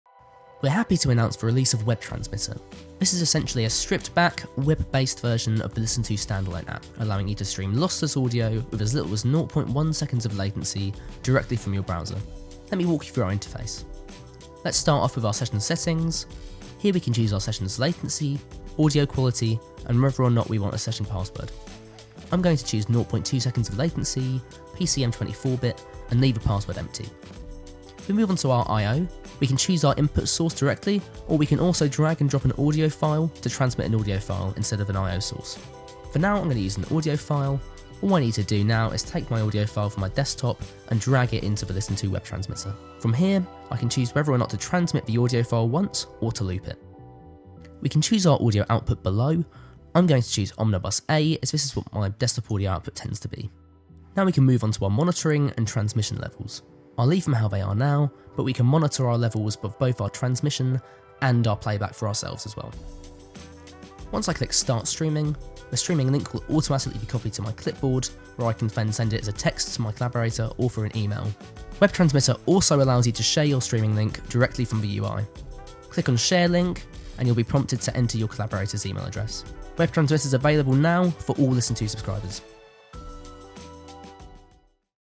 Announcing Web Transmitter — a unique, browser-based audio-sharing tool
We’re thrilled to announce the release of Web Transmitter — a unique, browser-based audio-sharing tool. Web Transmitter is a simplified, web-based version of the LISTENTO app you can launch straight from the browser

Web Transmitter allows you to stream lossless audio (up to 32 bit PCM) with as little as 0.1 seconds of latency directly from your browser.

Our product specialist, Eddie Pitman, walks you through the new interface here: from controlling session settings, the drag and play option and how you can share your streaming link directly from the user interface.